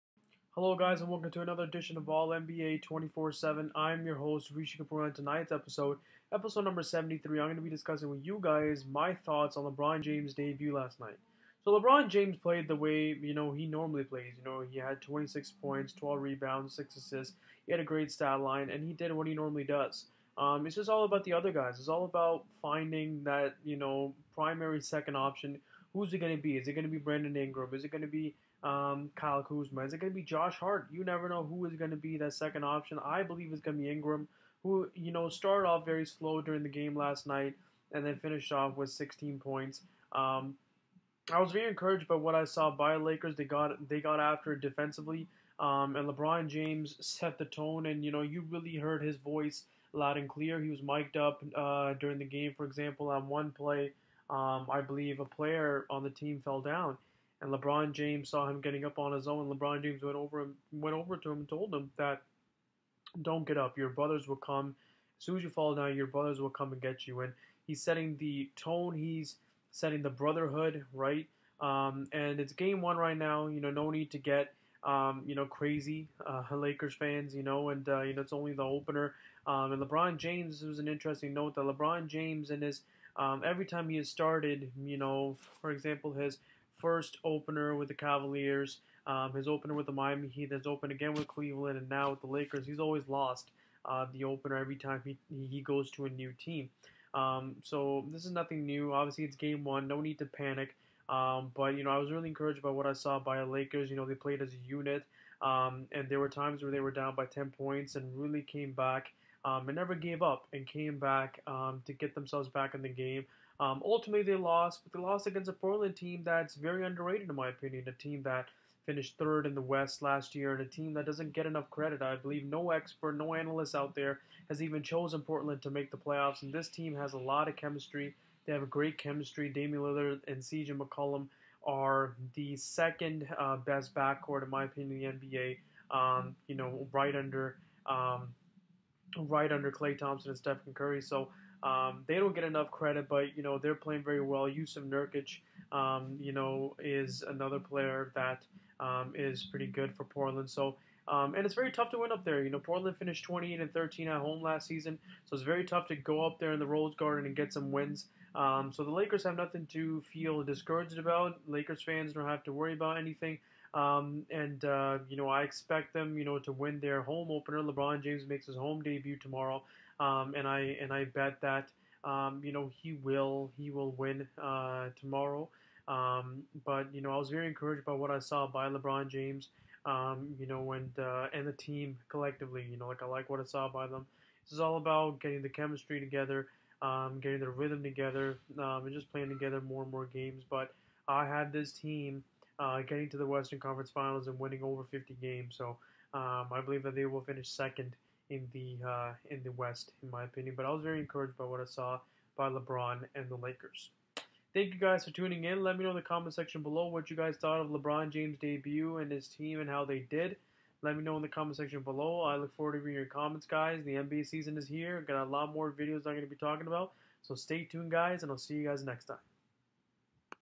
LEBRON JAMES DEBUT RECAP
In episode 73 I discuss LeBron's Laker debut